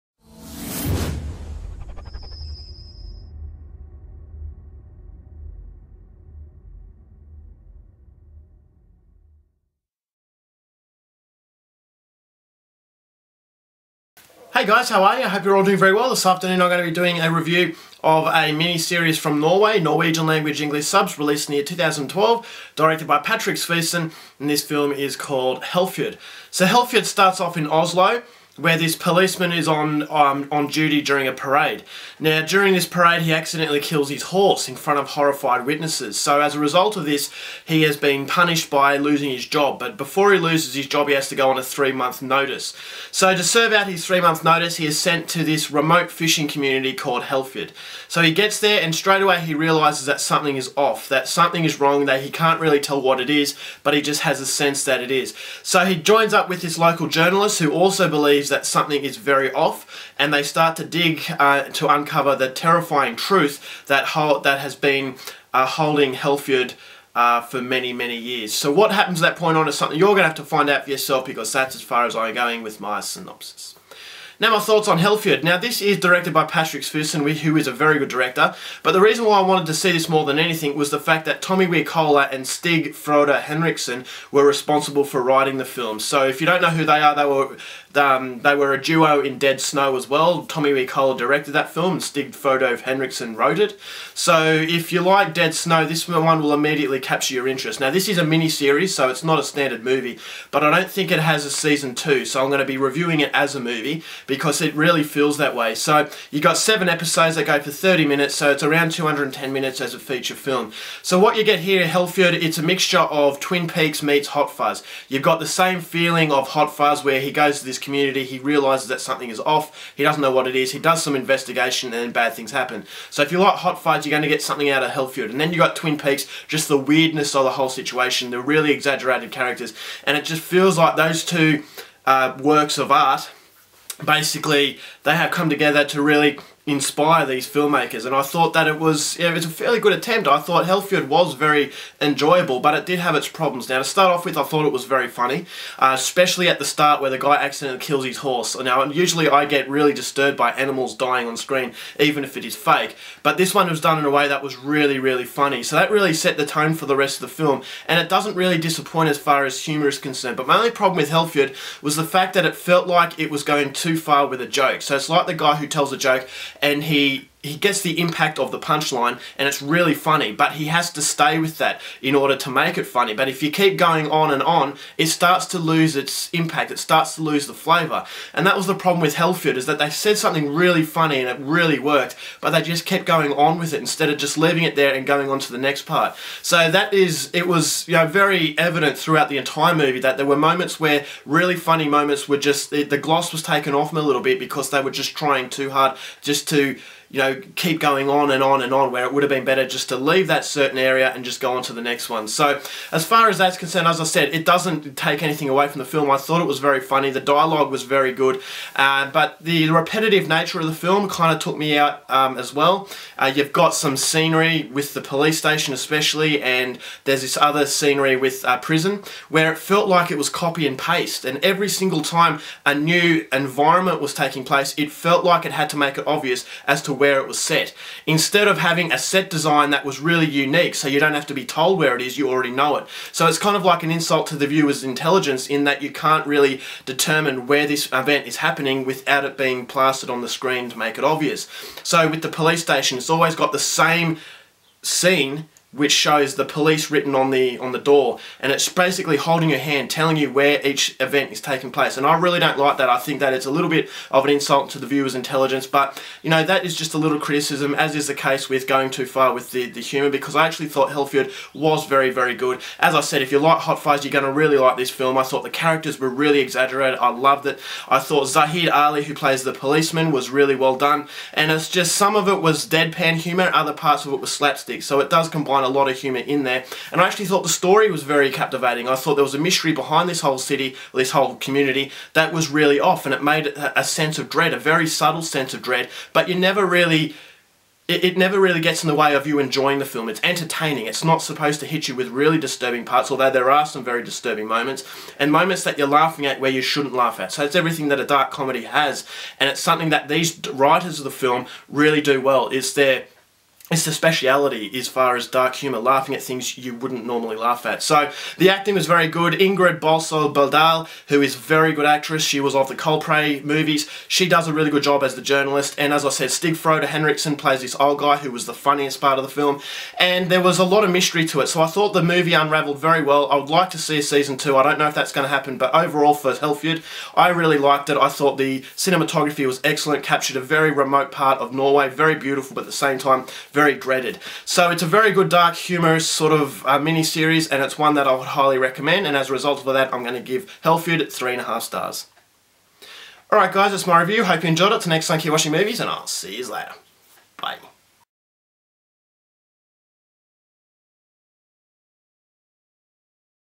Hellfjord (2012) Movie Review
After accidentally killing his horse in front of many horrified parents and children, Salmander, a Police Officer, is sacked and given his three months notice. While serving out his notice, he is sent to a remote fishing community called Hellfjord, as punishment. Upon his arrival, he notices that something is off with this seemingly peaceful existence. After doing a bit of detective work, with the help of the local journalist, Salmander uncovers the sinister secret that has plagued Hellfjord all this time, but the closer he gets to the truth, the more dangerous the situation becomes!